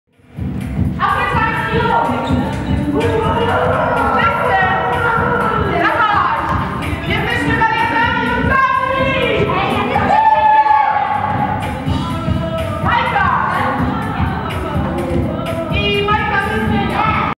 Idzie Młodość. Lato w teatrze +. Praca wre !
To wszystko dzieje się dziś - dziewiątego dnia pracy warsztatowej - w grupie dziecięcej. Jest moc !